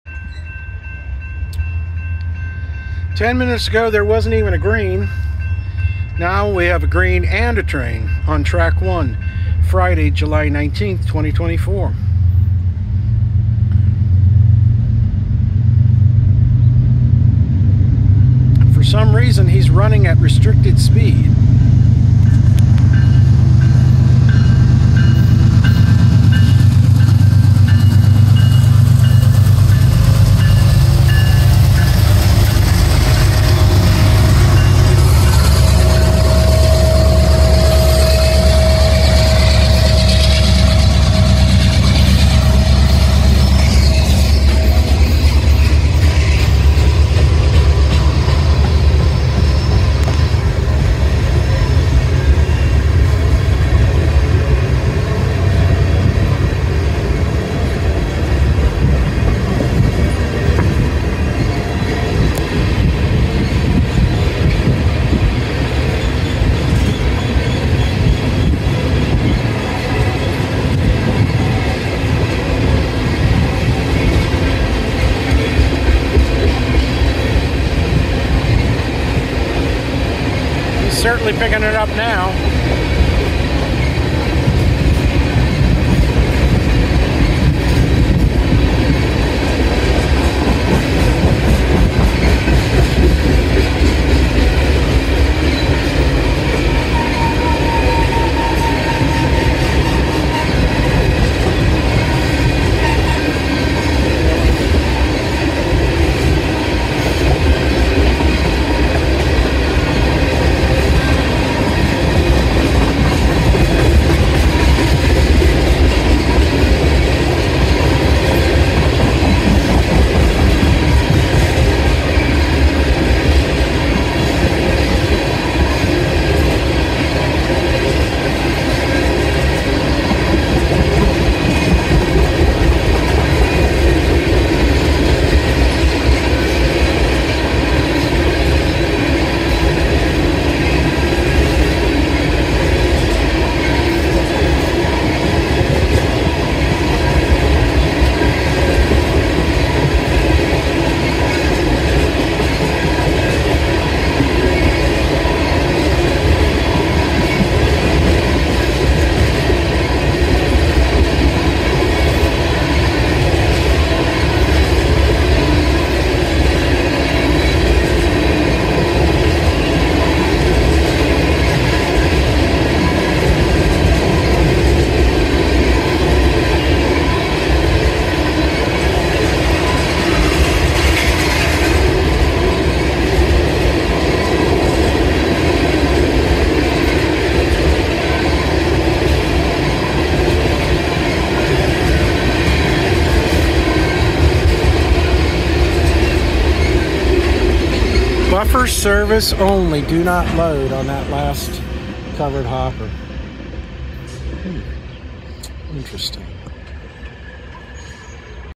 Northbound CSX empty ethanol tanks on track one, crossing Sharon Avenue
Glendale, Ohio Friday, July 19th, 2024 about 1:52 pm